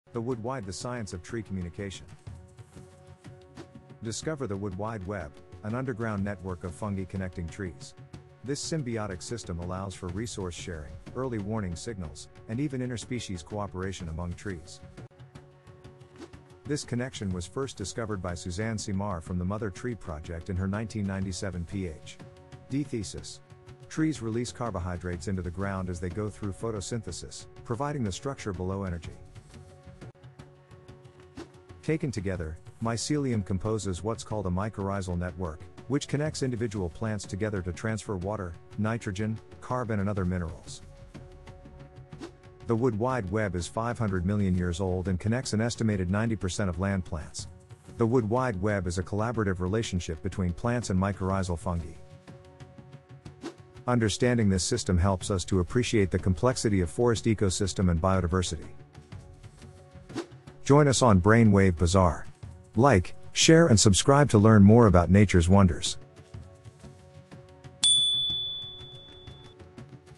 Wood Wide Web: How Trees Communicate Beneath Our Feet",#1111
Discover the fascinating underground network connecting trees and plants in the forest! In this short video, learn how trees communicate, share resources, and support each other through their roots and mycorrhizal networks. Dive into the secrets of the Wood Wide Web!"